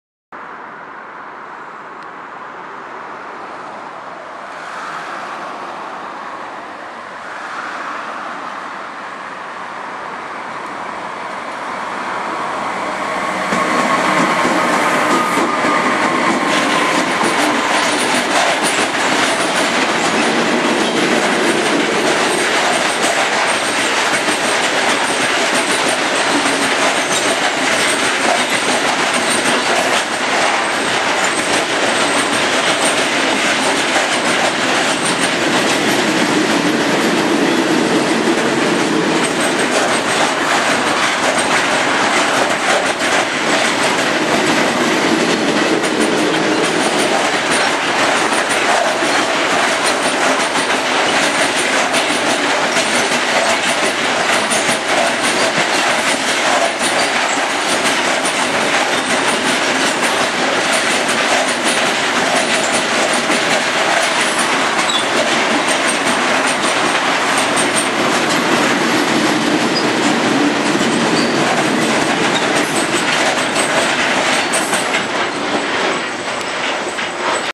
AK 189 031 - 8 + AK 189 046- 6 in Dillingen-Saar!
Die AK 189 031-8 + AK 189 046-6 mit einem Erzzug durchfährt den Bahnhof von Dillingen-Saar zur Dillinger-Hütte!